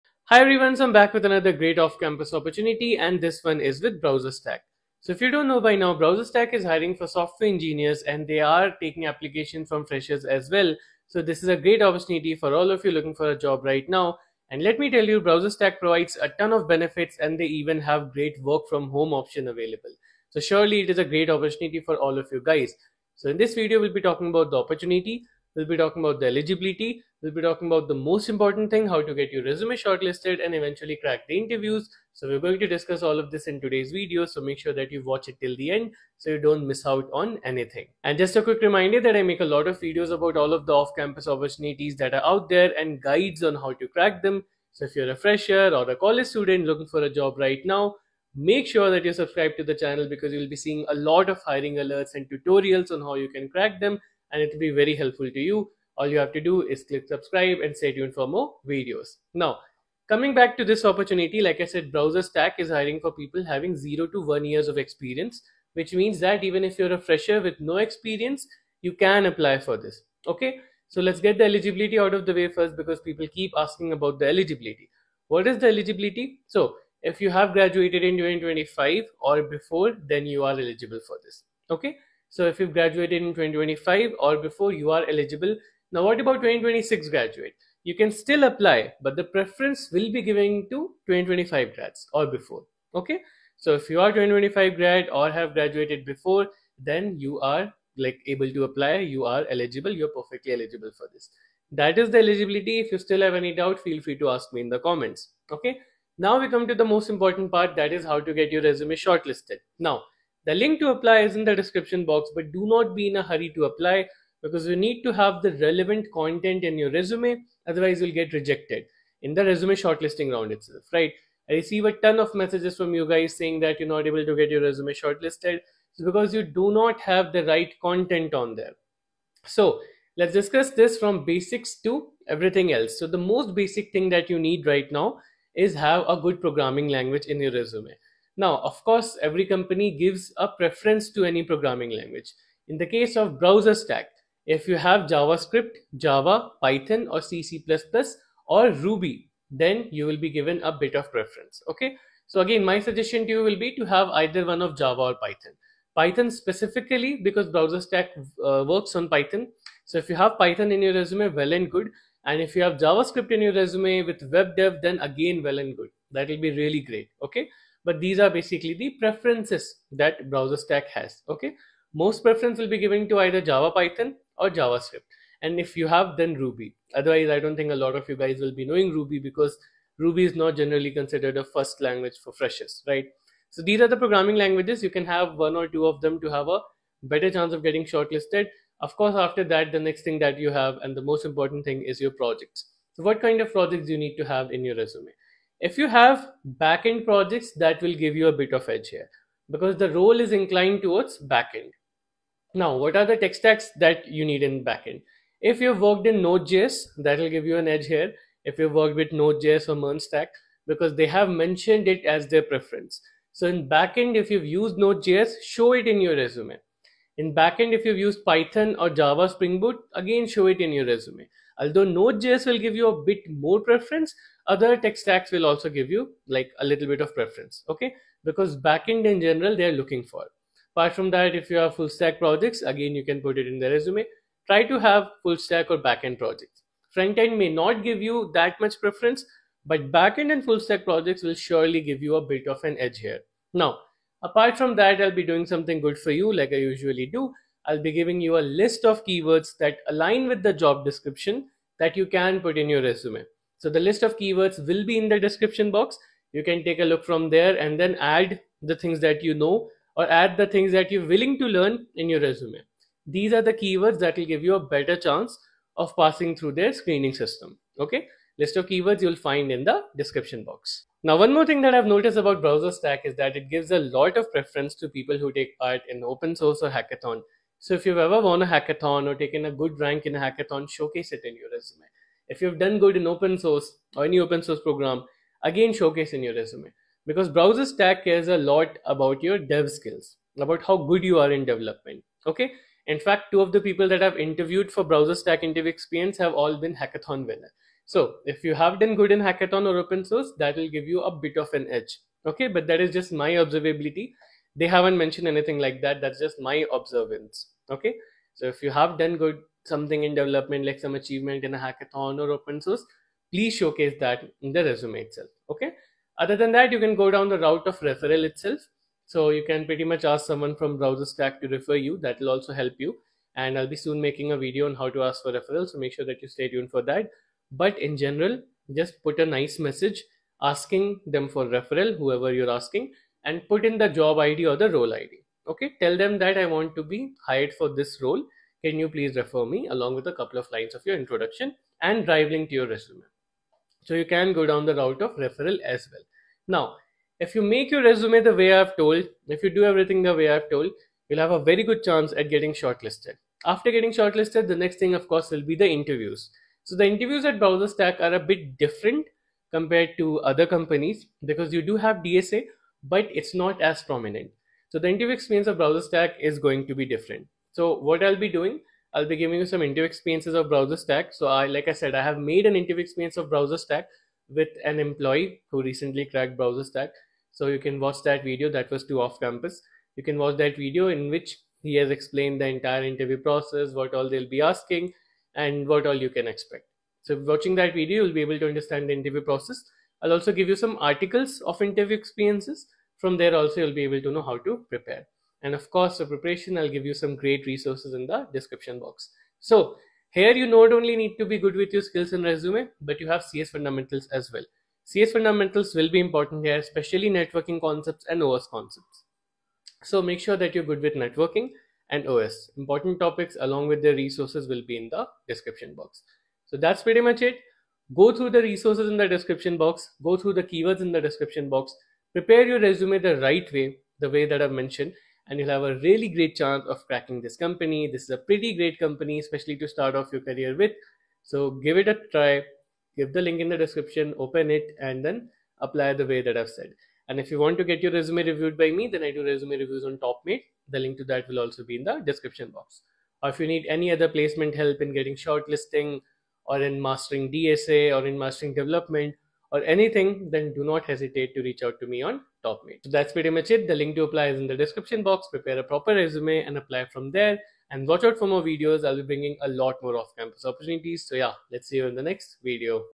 BrowserStack Hiring Freshers🔥Apply Now | Work From Home
00:00 Introduction
01:10 Eligibility
01:44 Resume Shortlisting
05:46 Interviews